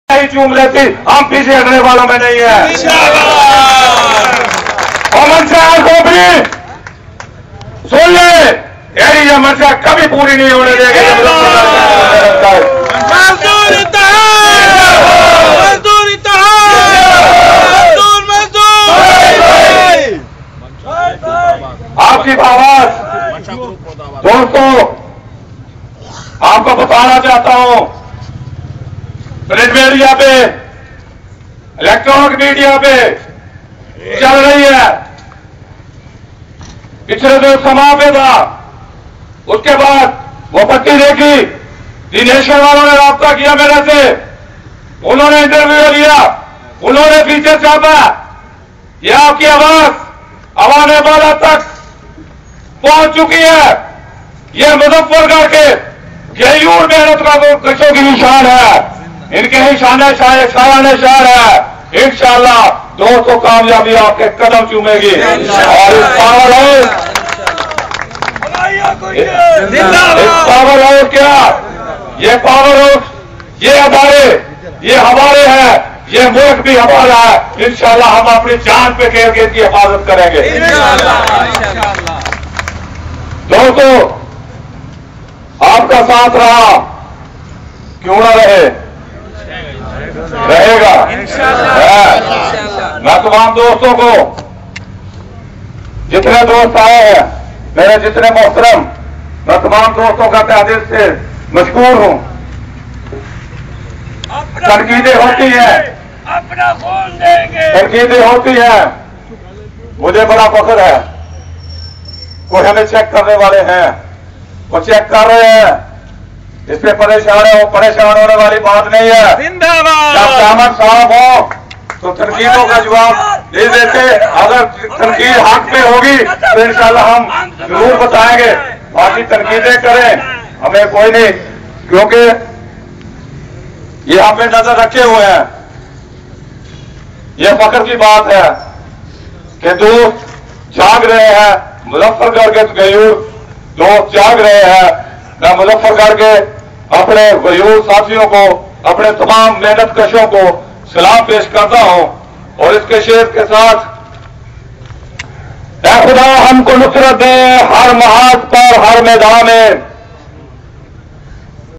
Genco-3 (Hamyyun Masood Gujar)Part-36_clip10.flv
Hamyyun Masood Gujar (Regional Chairman) Speech Against Wapda Privitization.
(Thermal Power Station MuzaffarGarh Strikes).